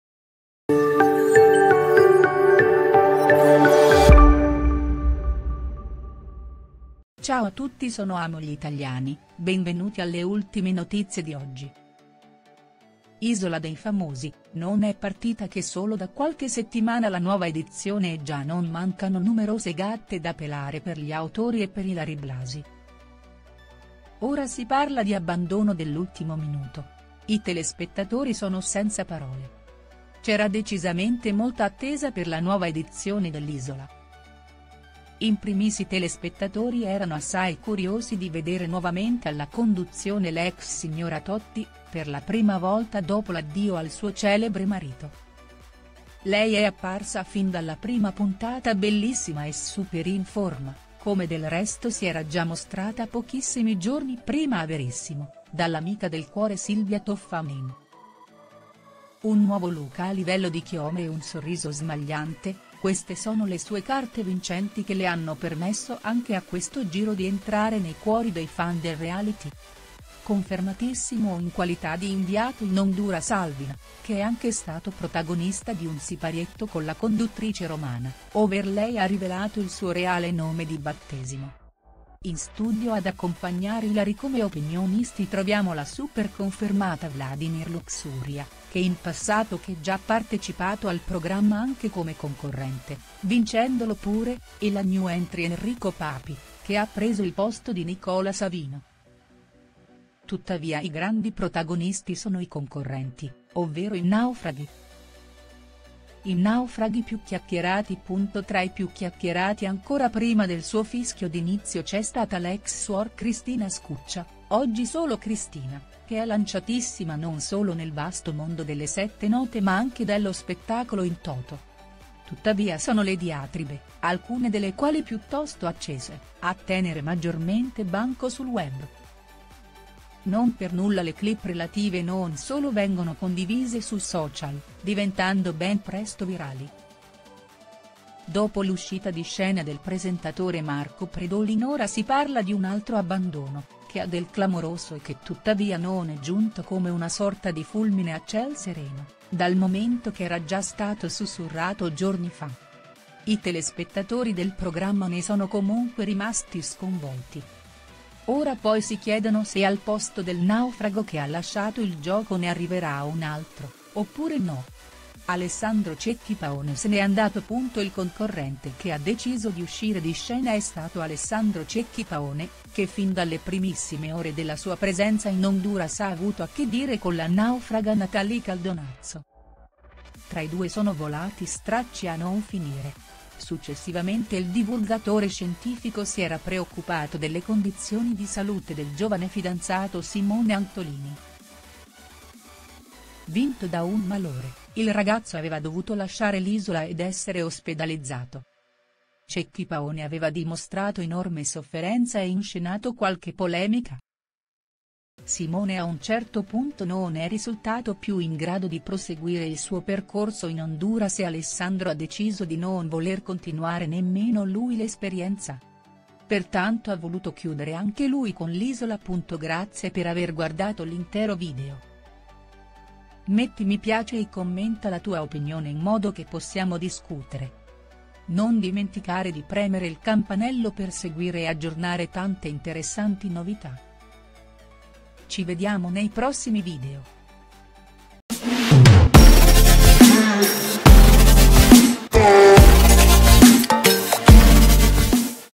Isola dei famosi, abbandono dell’ultimo minuto il disastro preannunciato giorni fa
C’era decisamente molta attesa per la nuova edizione dell’Isola. In primis i telespettatori erano assai curiosi di vedere nuovamente alla conduzione l’ex Signora Totti, per la prima volta dopo l’addio al suo celebre marito. Lei è apparsa fin dalla prima puntata bellissima e super in forma, come del resto si era già mostrata pochissimi giorni prima a Verissimo, dall’amica del cuore Silvia Toffanin.

Un nuovo look a livello di chiome e un sorriso smagliante, queste sono le sue carte vincenti che le hanno permesso anche a questo giro di entrare nei cuori dei fan del reality. Confermatissimo in qualità di inviato in Honduras Alvin, che è anche stato protagonista di un siparietto con la conduttrice romana, over lei ha rivelato il suo reale nome di battesimo.